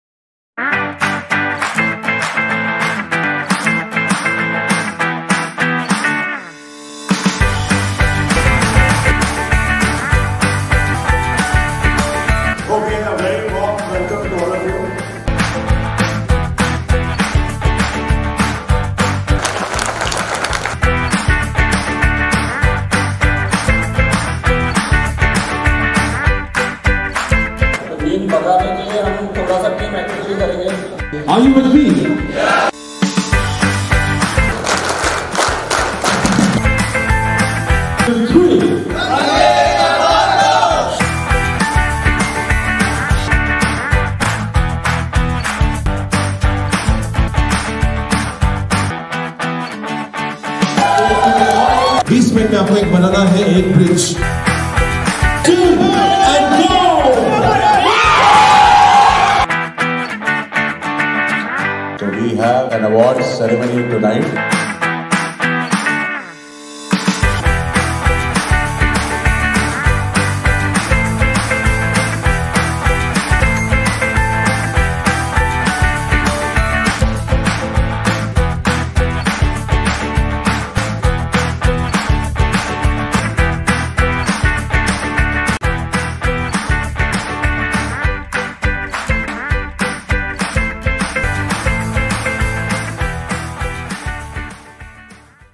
Distributor Meet - Center and West Chapter
FFPL's Center and West chapter Annual Distributor Meet (2023) was organized recently. The meet was a great occasion for our distributor partners to come together and have an evening of fun and talk. Here's to another year of success and growth with them!